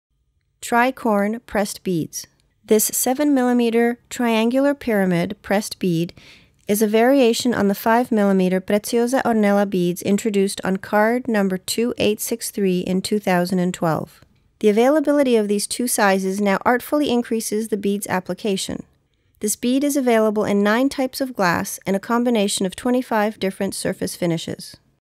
PRECIOSA ORNELA SAMPLE CARD 2901
Tricorne - Pressed Beads
This 7 mm triangular pyramid pressed bead is a variation on the 5 mm PRECIOSA ORNELA beads introduced on Card No. 2863 in 2012.  The availability of these two sizes now artfully increases the bead's application. This bead is available in nine types of glass and a combination of 25 different surface finishes.